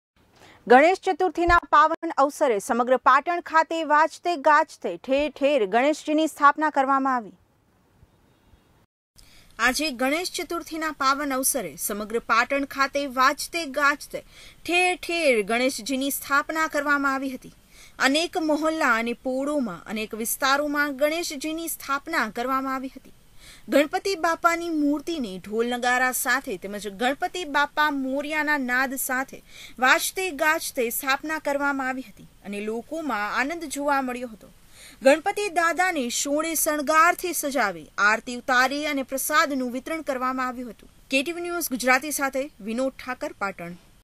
પાટણ-ગણેશ ચતુર્થીના પાવન અવસરે સમગ્ર પાટણ ખાતે વાજતે ગાજતે ઠેર ઠેર ગણેશજીની
પાટણ-ગણેશ ચતુર્થીના પાવન અવસરે સમગ્ર પાટણ ખાતે વાજતે ગાજતે ઠેર ઠેર ગણેશજીની સ્થાપના કરવામાં આવી...